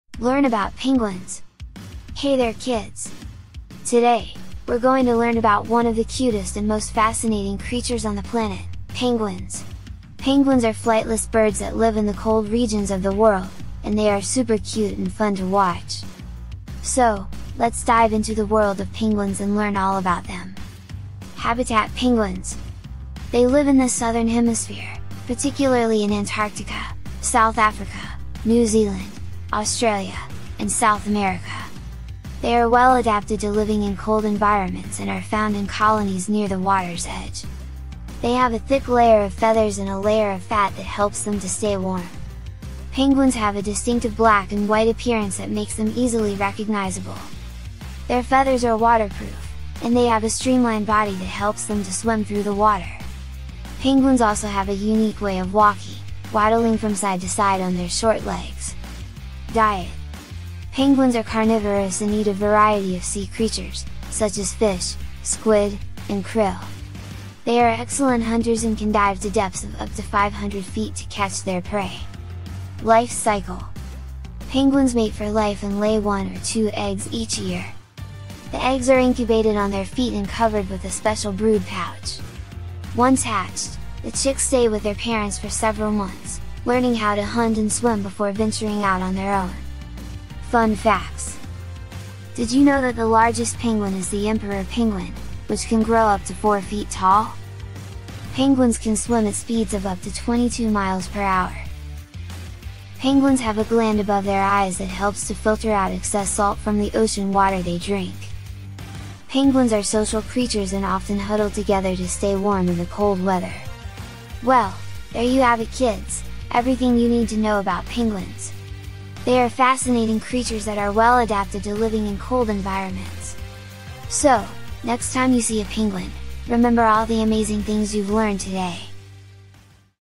Learn About Penguins
Penguins are flightless birds that live in the Southern Hemisphere. They are well adapted to living in cold environments and have a thick layer of feathers that keeps them warm. Penguins are excellent swimmers and can dive to depths of over 500 feet. They are carnivores and mainly eat fish and squid. Penguins live in large colonies and are social animals that communicate with each other through vocalizations and body language. There are 18 species of penguins in the world, ranging in size from the smallest, the fairy penguin, to the largest, the emperor penguin. Penguins are threatened by habitat loss, climate change, and overfishing.